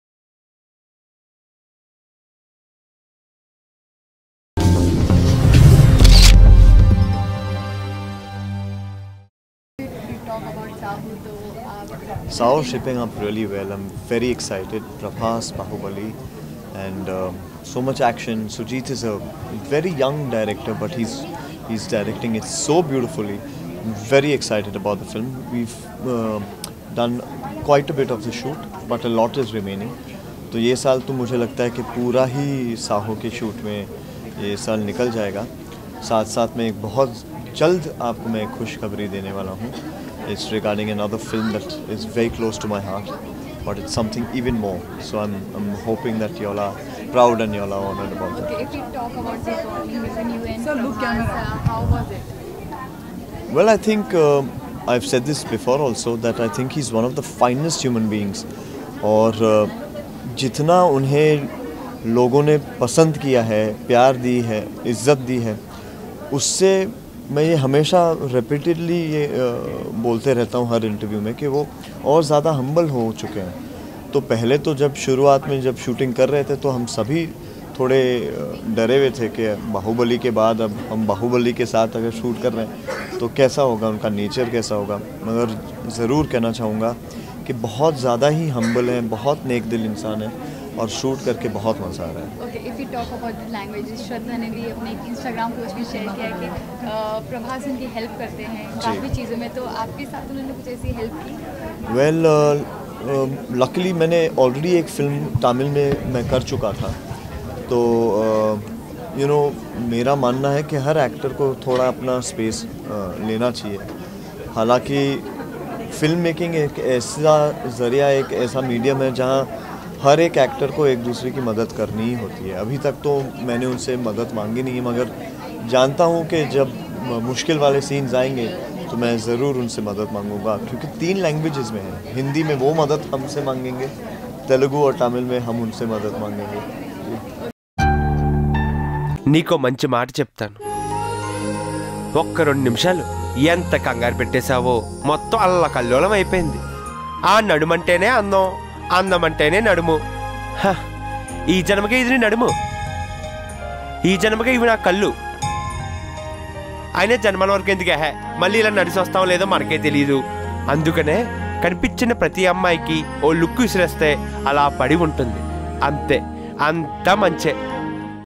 Neil Nithin Mukesh says I Was Very Scared to do Movie with Prabhas - Filmyfocus.com
Neil Nithin Mukesh says I Was Very Scared to do Movie with Prabhas. Saaho Movie Stars Prabhas, Shraddha Kapoor, Neil Nithin in Lead Roles, Directed by Sujith, Produced by UV Creations.